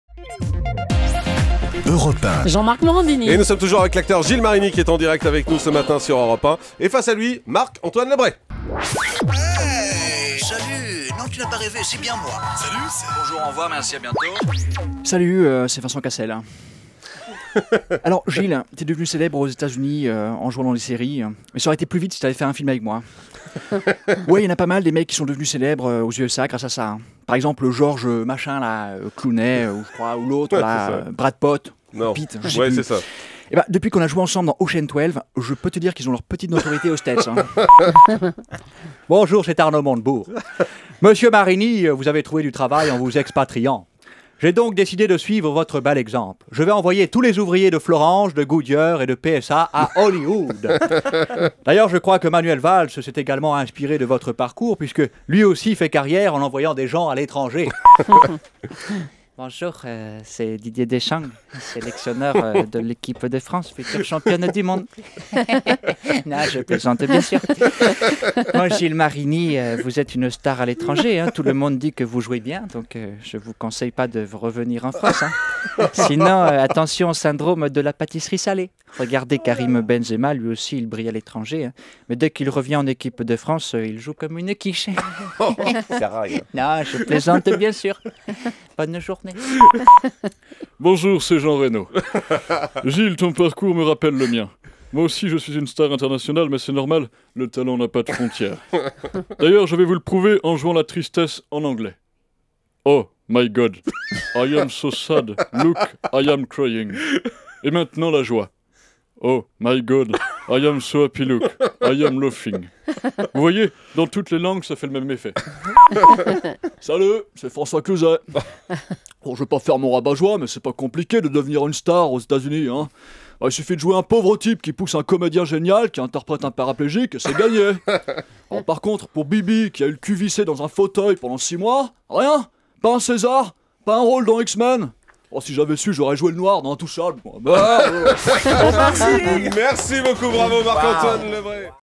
Célèbre comme Brad Pote
Vincent Cassel pense que Gilles Marini serait devenu célèbre bien plus rapidement s'il avait joué dans ses films.